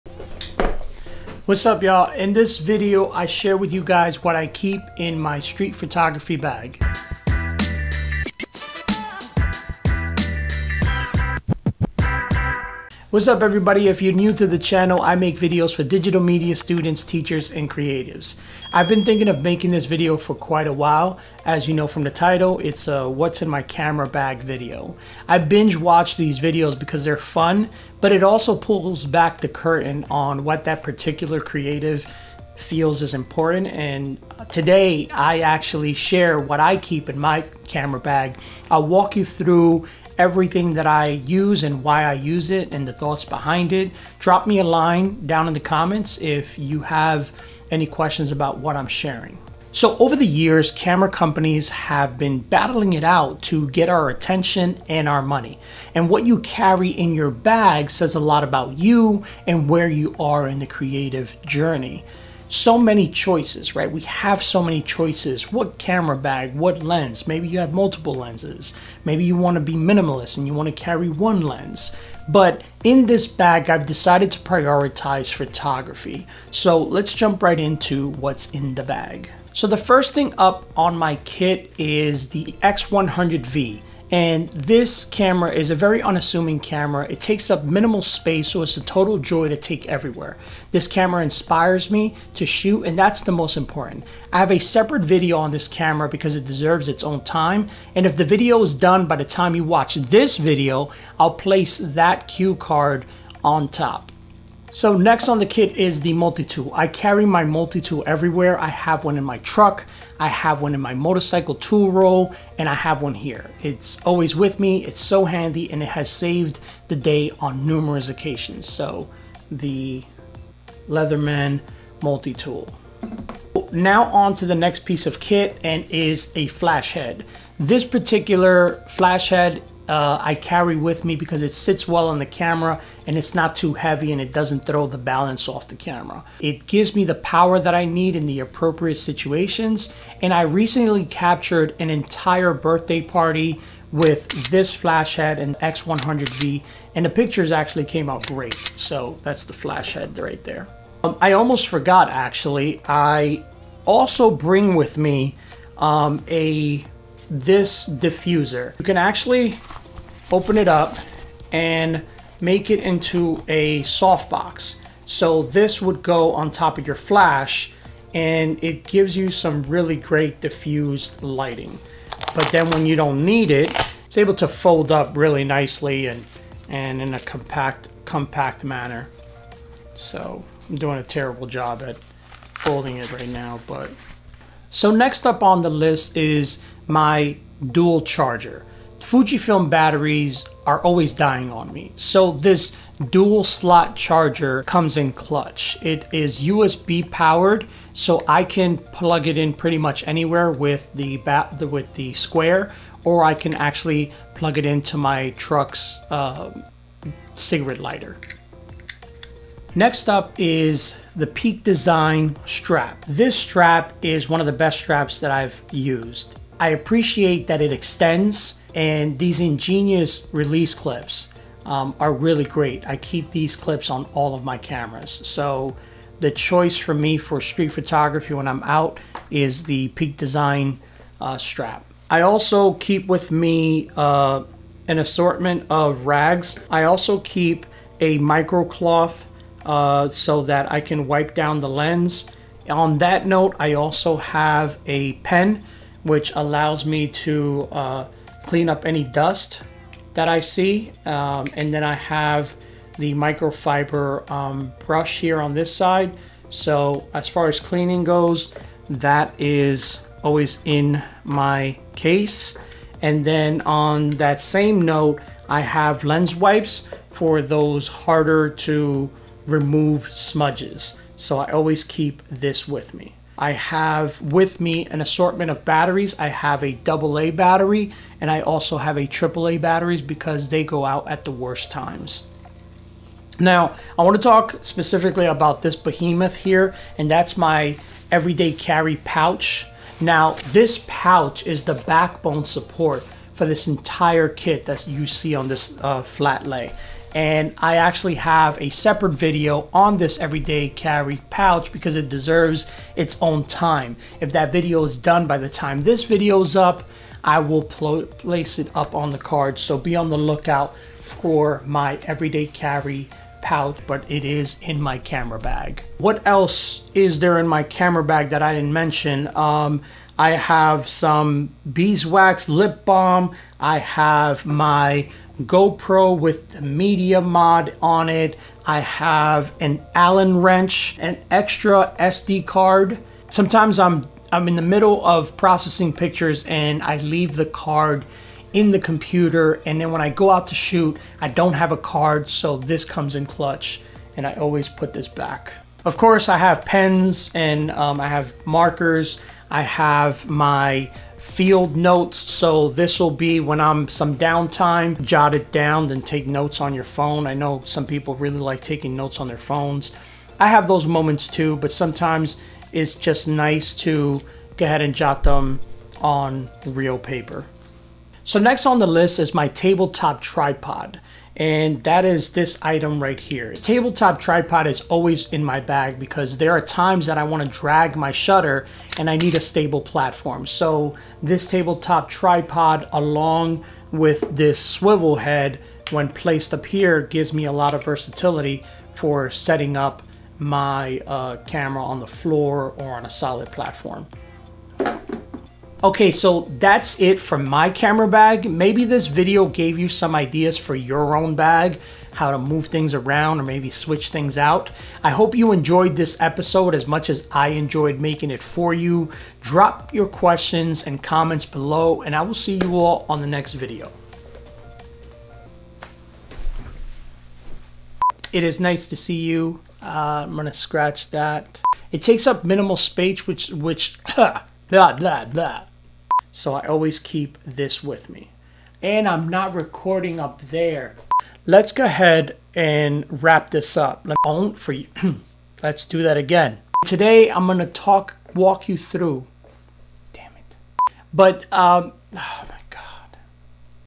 20. What's in my camera Bag 2023
I discuss what is in my camera bag while on the streets doing one of my favorite things, street photography.

Timestamps
0:00 Welcome
0:07 Show Bumper clip
0:12 Video purpose
0:50 Video's why
1:19 The Camera
1:45 Multi-tool
2:05 Flash head
2:33 Flash soft box
3:08 Fujifilm battery charger
3:30 Camera strap
3:50 Rag
4:04 Brush pen
4:20 Lens wipes
4:33 AA and AAA
4:43 EDC pouch
5:16 what else?
5:23 GoPro with media mod
5:30 Allen wrench
5:32 SD card
5:40 Field notes
6:08 Table-top tripod
6:36 Outro
6:58 Popular demand: bloopers